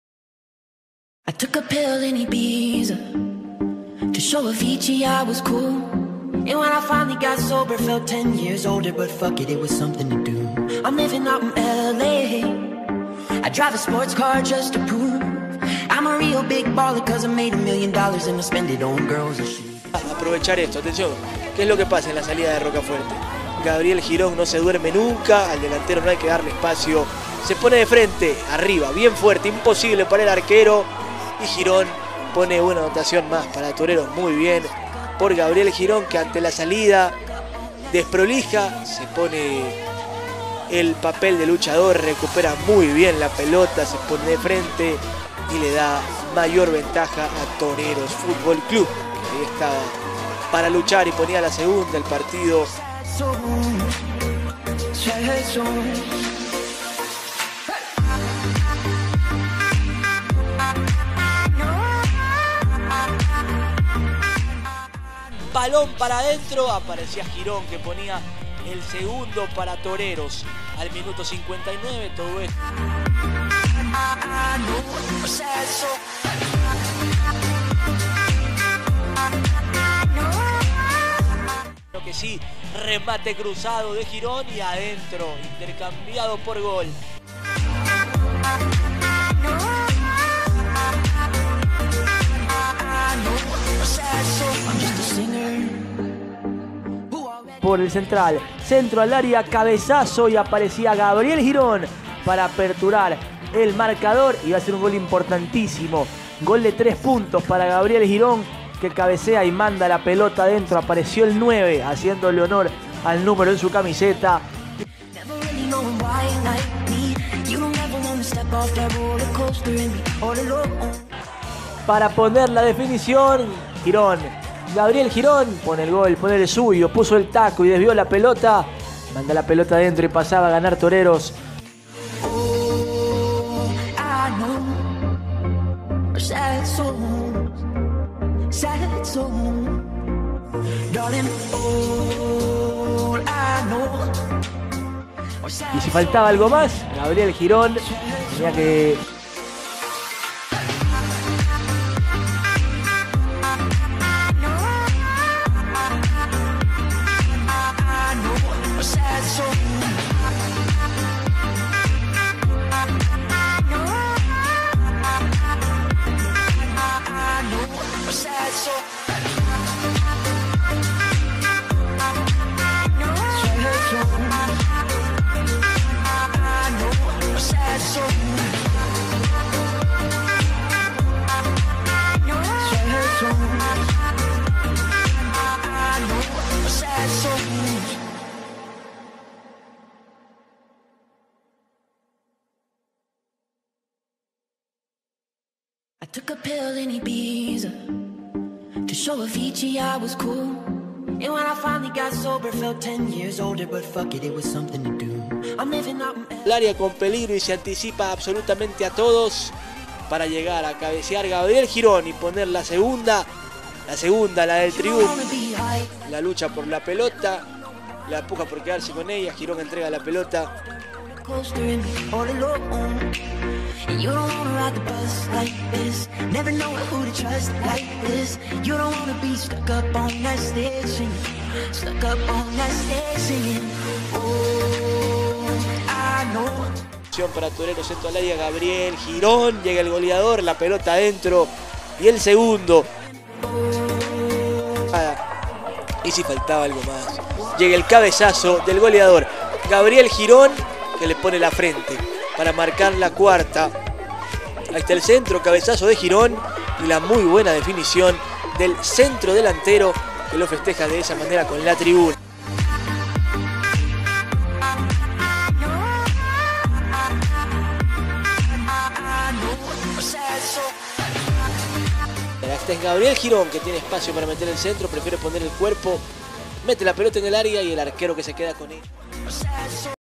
GABRIEL GIRÓN DELANTERO
Gabriel Girón centro delantero, buen juego aéreo y potencia, año 2001, club actual Barcelona SC. agencia de representación del jugador FranMaScouting con sede en Usa- Ecuador - Francia e Inglaterra. email para contactos